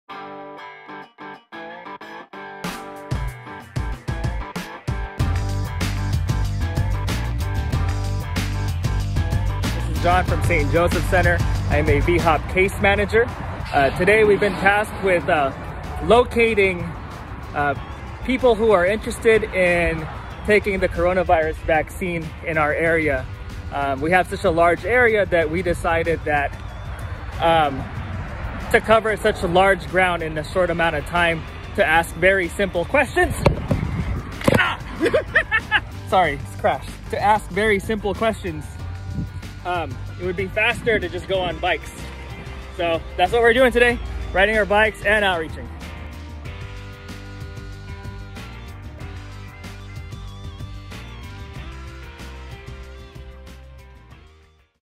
VHOP Ventures: 11 People Living On the Street Get Vaccinated
St. Joseph Center's Vehicular Homeless Outreach Program (VHOP) is very creative in doing outreach. Recently, VHOP had to quickly get the word out about more vaccines being available to people experiencing homelessness.

So, they took their bikes.

While collaborating with the Department of Health and peer-agency, HOPICS, VHOP helped 11 people living on the street get vaccinated.